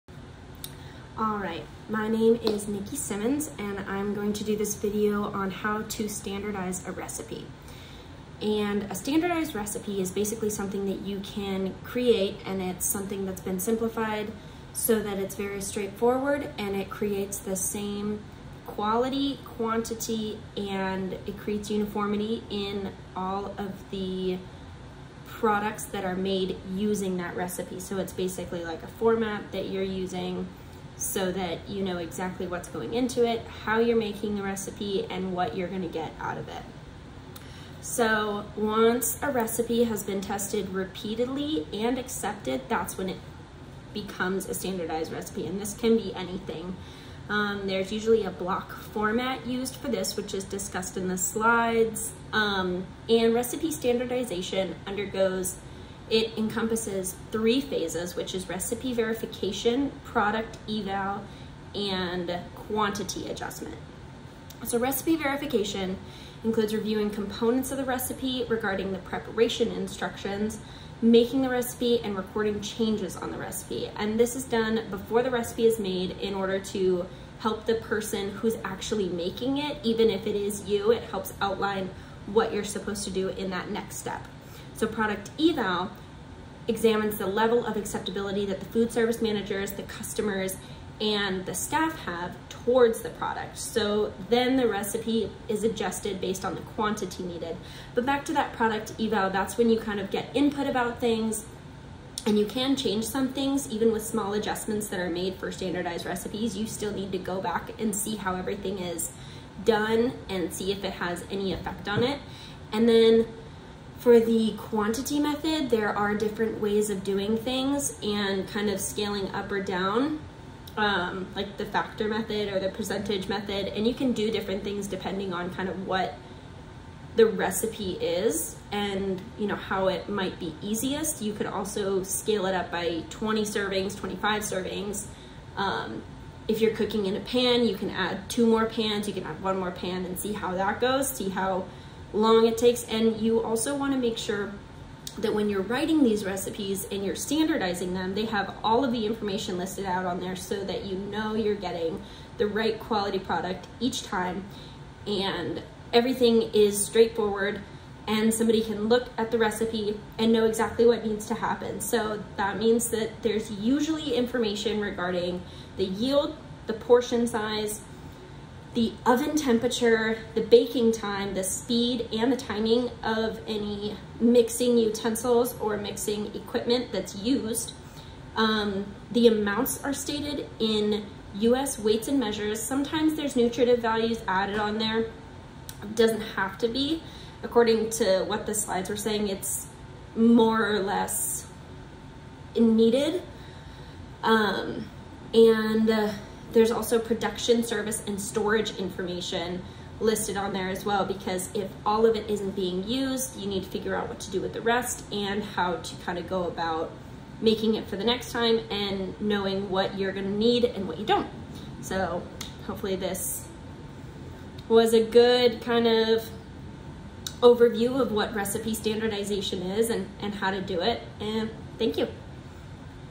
How to Standardize a Recipe FSN 344
This video explains the basic steps for standardizing recipes in the foodservice industry and what is needed to do so.